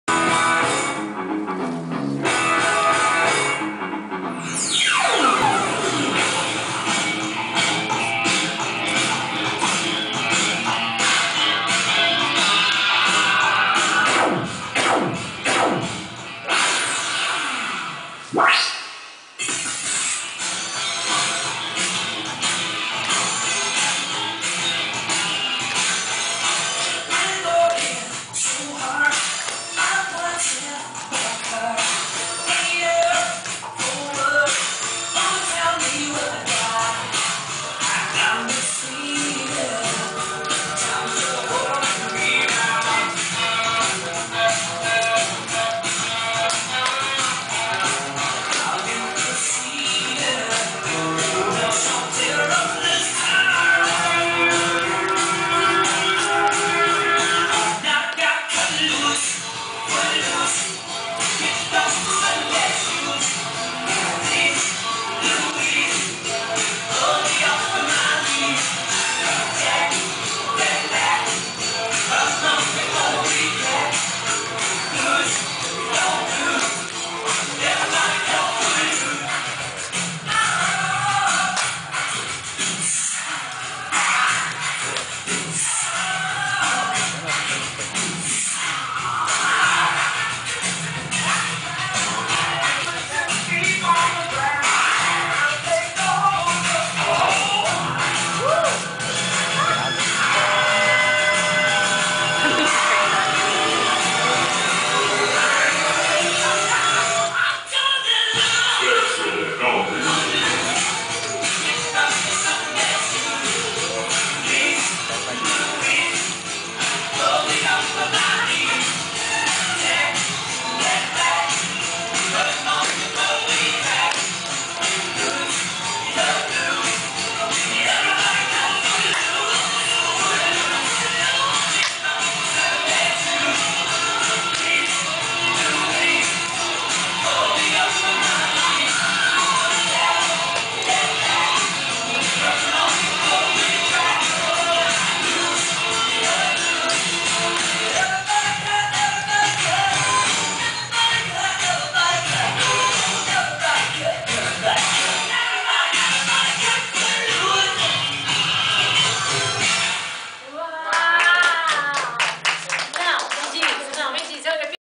Acacio Sampaio no Mulheres Dez
Apresentação de dança country - Acácio Sampaio da Flexion Academia, de Cubatão, no programa Mulheres Dez, transmitido pela Santa Cecília TV para toda região metropolitana da Baixada Santista. Apresentadora: Isabel Machado